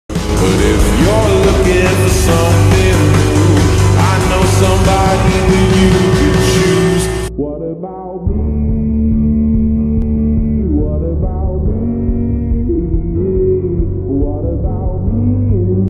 What about me?.. [ Au Description I’m desc ]
Kira and Rowen started to date
But Loren developed feelings for Kira
And acts happy for them